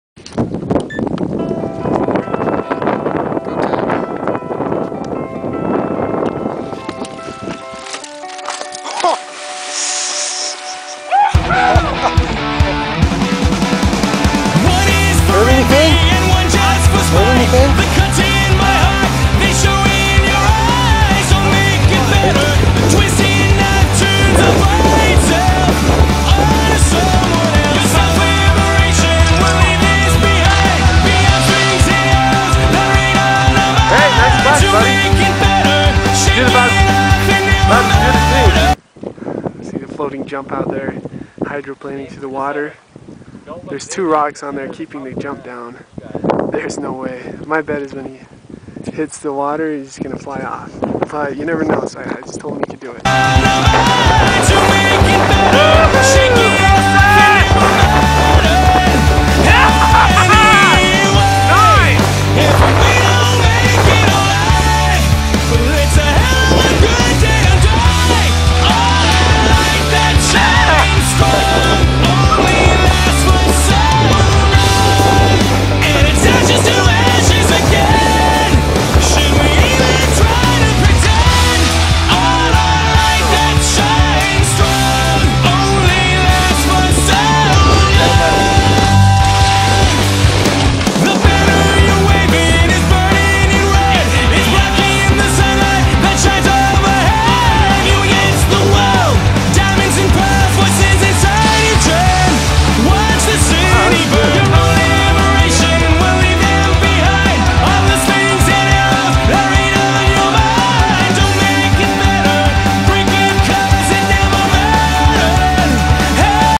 Mona Reservoir Cliff Jumping Mountain Bikes
Scott And Seth Jumping Mountain Bikes Into Mona Reservoir.